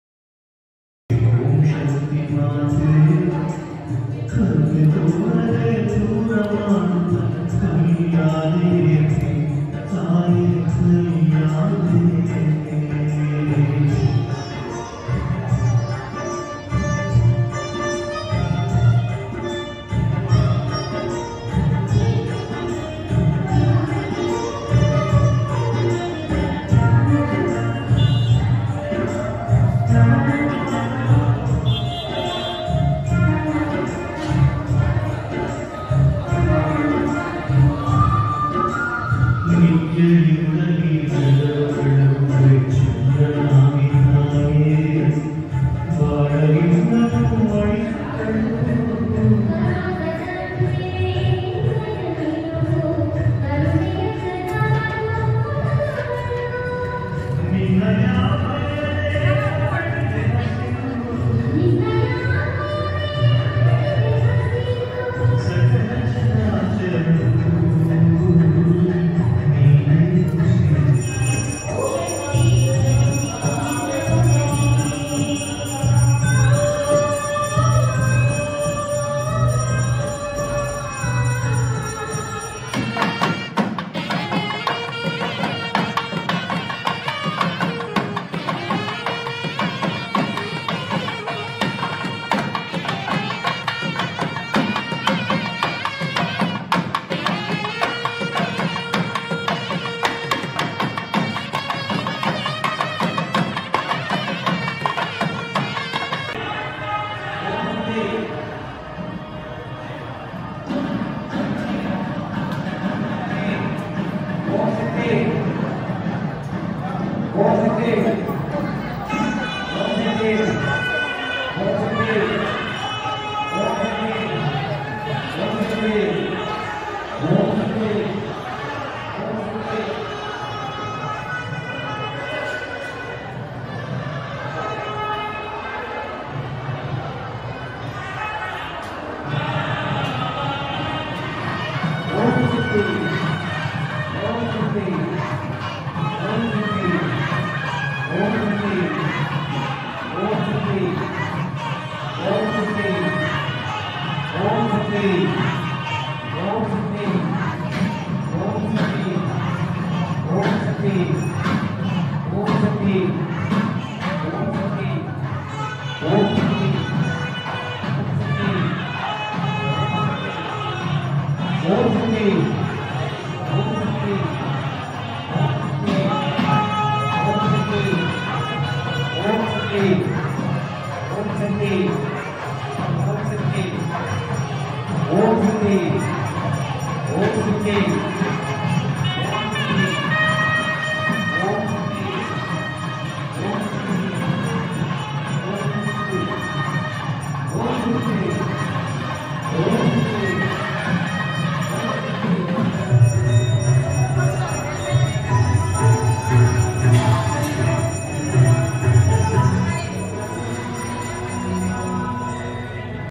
🕉️ ஓம் சக்தி கோவிலில் பால் அபிஷேகம் ஓம்சக்திகோவில் ஓம்சக்திபராசக்தி பால்அபிஷேகம் livevideo 🙏🙏
ஓம் சக்தி கோவிலில் பால் அபிஷேகத்தின் நேரடி வீடியோவைப் பார்த்து பகிரவும்
ಓಂ ಶಕ್ತಿ ದೇವಸ್ಥಾನದಲ್ಲಿ ಬಾಲ ಅಭಿಷೇಕದ ಲೈವ್ ವೀಡಿಯೊವನ್ನು ವೀಕ್ಷಿಸಿ ಮತ್ತು ಹಂಚಿಕೊಳ್ಳಿ
Watch and share the live video of Baal Abhishekam at Om Shakti Temple